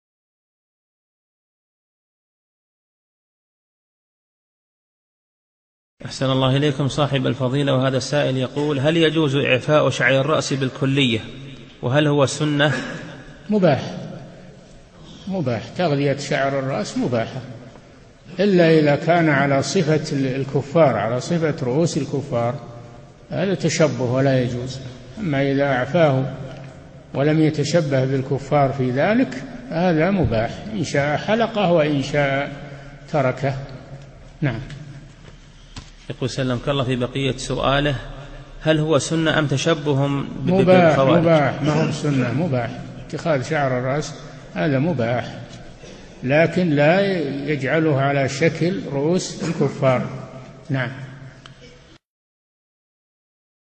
A lejohet zgjatja e flokëve Shejh Salih el Feuzani
A lejohet zgjatja e flokëve Shejh Feuzani
Shejh i nderuar, pyetësi thotë: A lejohet zgjatja e flokëve dhe a është ky veprim prej Sunetit?
Shejhu i nderuar Salih el Feuzani (Allahu e ruajttë!): Është mubah (e lejuar), mubah. Zgjatja e flokëve është mubah (e lejuar). Përveç nëse zgjatja e flokëve është (bëhet) në atë formë që i mbajnë dhe jobesimtarët, ky është përngjasim dhe nuk lejohet. Nëse personi i zgjat flokët duke mos u përngjasur me jobesimtarët në këtë gjë (nuk ndjek ndonjë model të tyre) kjo është mubah. Nëse dëshiron i rruan (shkurton) flokët e nëse dëshiron i lëshon (zgjat)
Pyetësi: Ka mbetur dhe një pjesë nga pyetja: A është sunet apo përngjasim…
Shejhu: Është mubah, mubah dhe nuk është sunet. Është mubah. Zgjatja e flokëve është mubah, por jo t’i mbash në atë formë (model) që i mbajnë dhe jobesimtarët.
Përktheu: Unejs Sheme